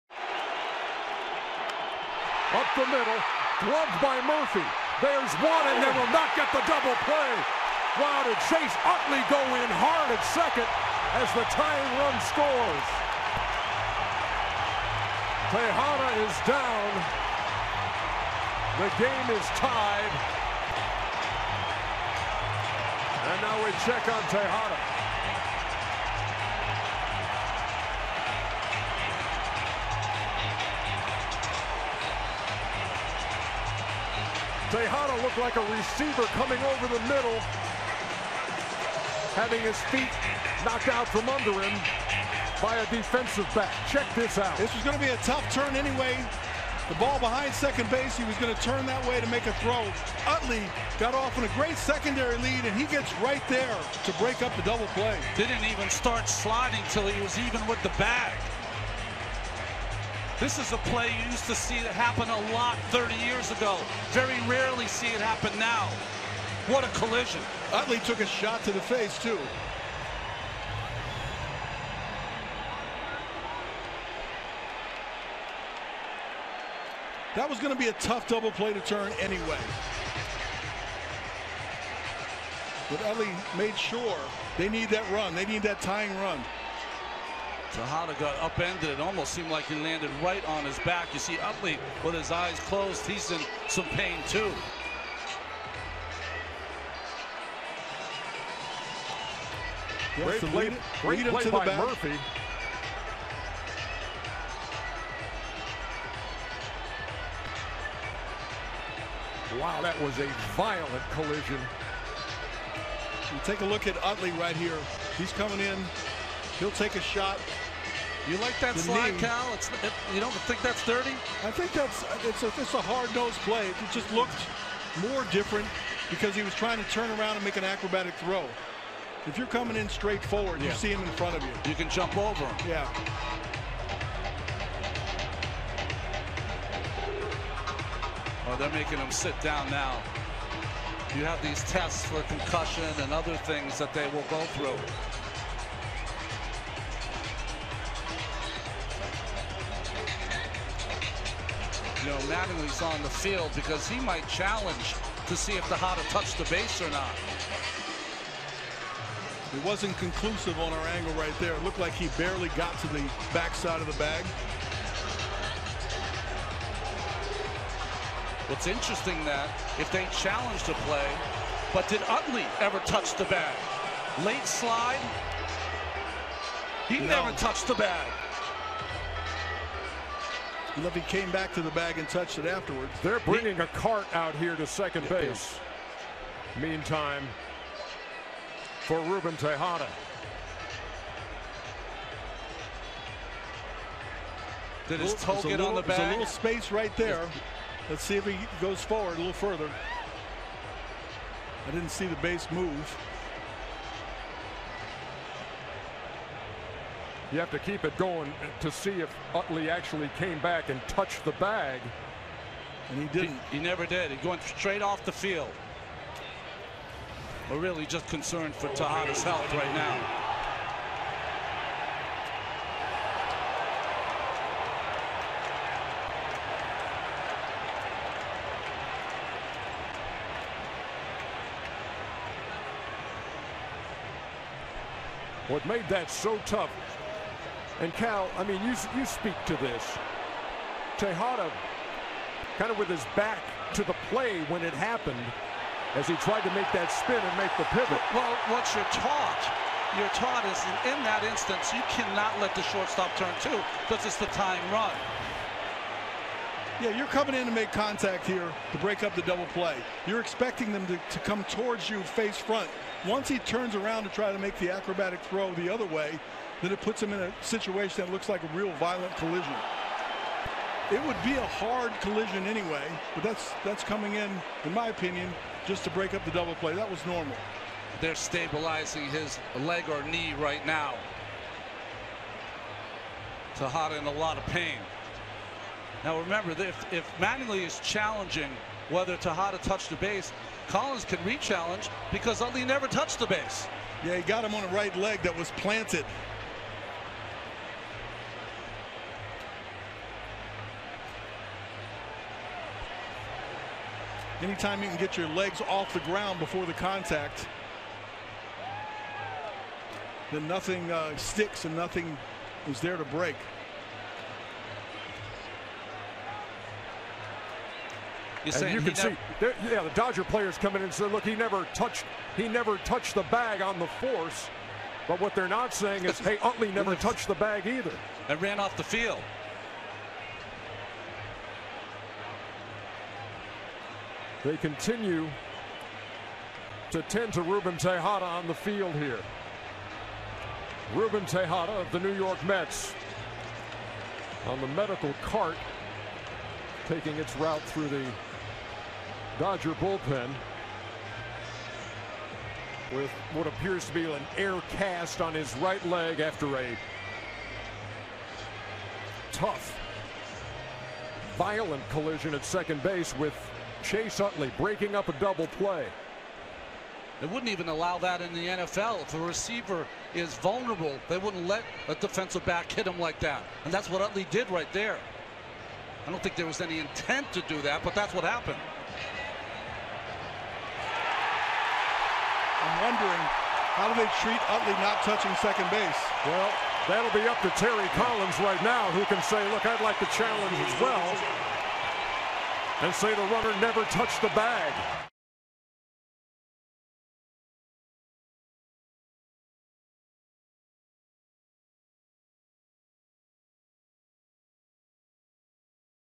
Kendrick drives in game-tying run in 7th
10/10/15: Howie Kendrick knocks in a run to tie the game at 2 after Chase Utley's hard slide breaks up a potential double play in the 7th